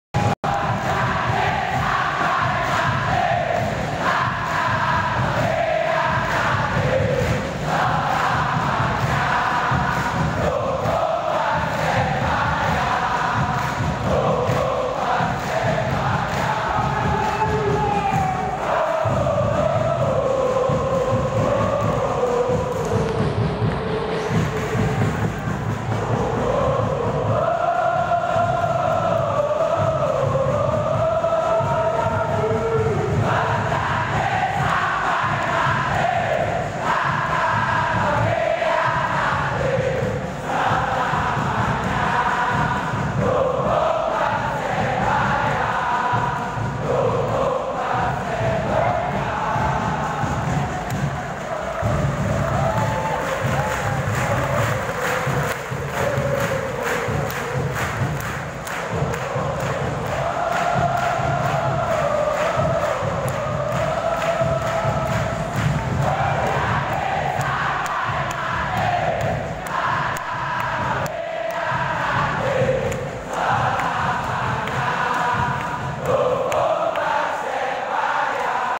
DAHLAN ISKAN | Spirit Juang Bonek Dukung PERSEBAYA VS PERSIB di Jalak Harupat
Pertandingan berakhir PERSEBAYA 3-2 PERSIB